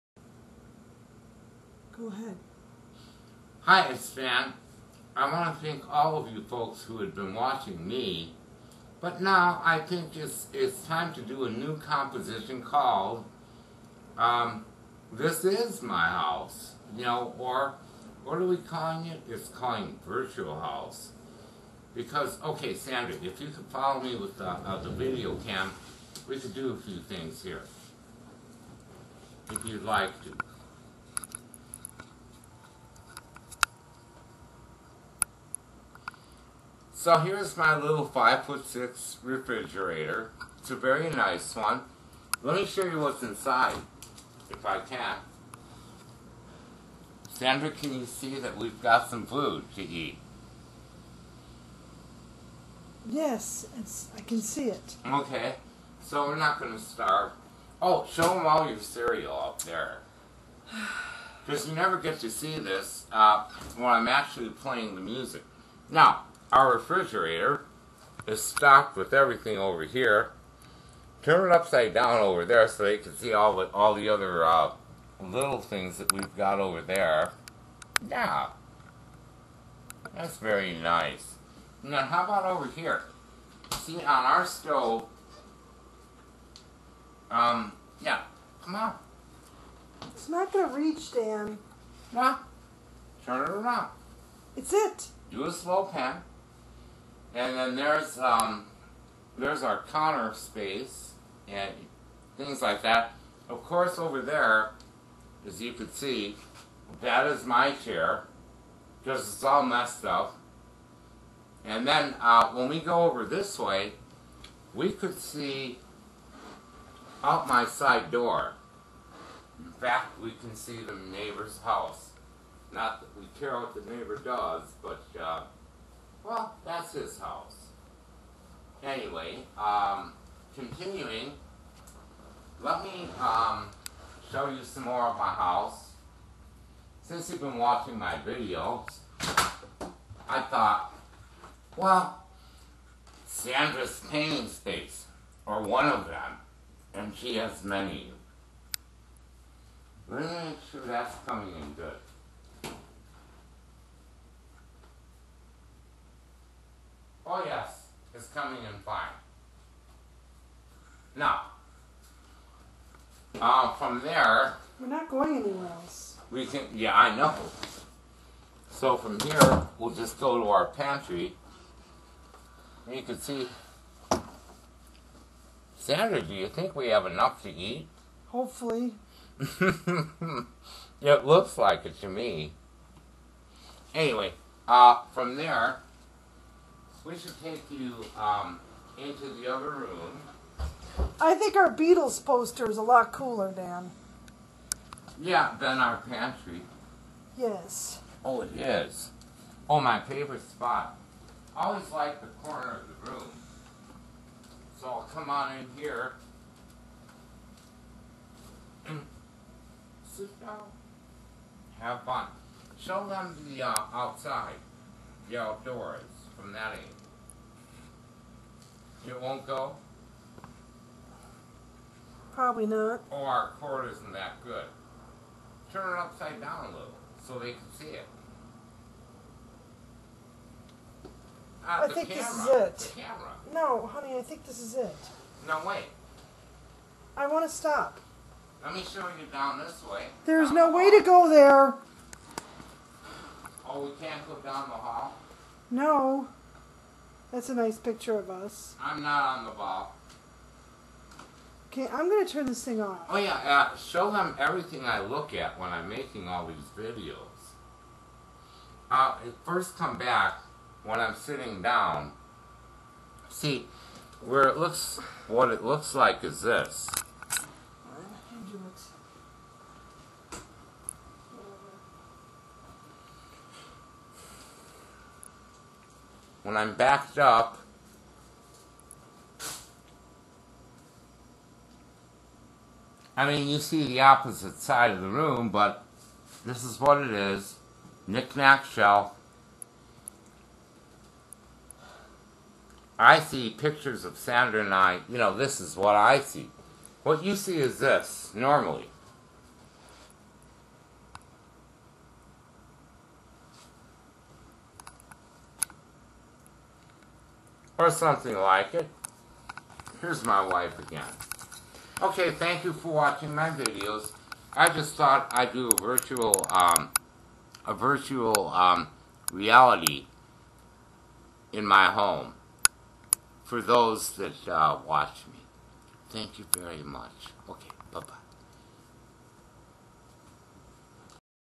our virtual home
Hello Guys, Dan wanted you to see parts of our humble home, what we get to see when you see us! Hello to all across the pond! Hope you are all well. Dan is a music teacher, and will be starting lessons soon, next month in Oct. Please check out for more on Dan's music. With special thanks to Sandra for making this video for me.